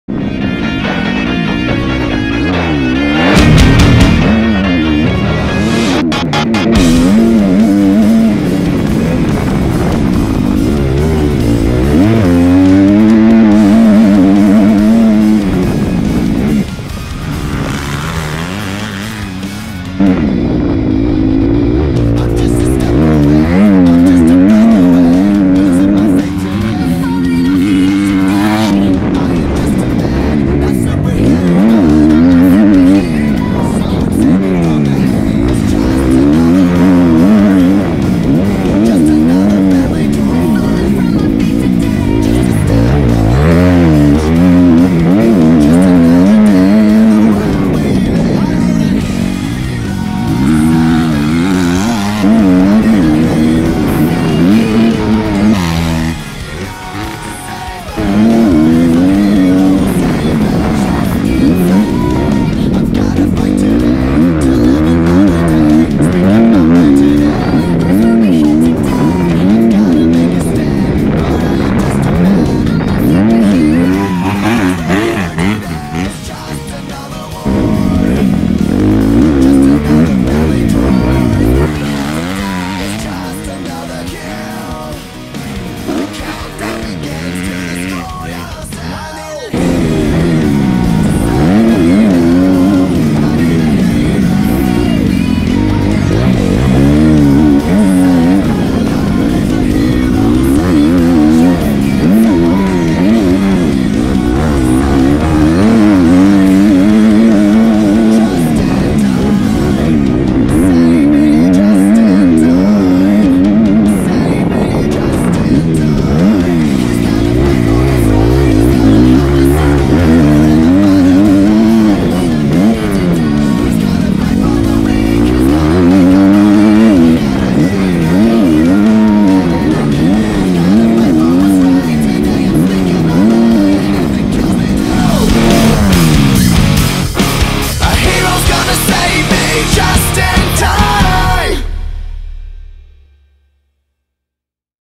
One lap with Mathias Jorgensen on stock Kawasaki 2019 250F at GAME MOTO Oklahoma
Hello Welcome back! While practice supercross here at GAME MOTO Oklahoma I decided to try out my Girlfriends 2019 Kawasaki Kxf 250 Complete stock bike. She wasnt too happy about the idea, so I promised to be a sweetheart and here is how it turned out. Cheers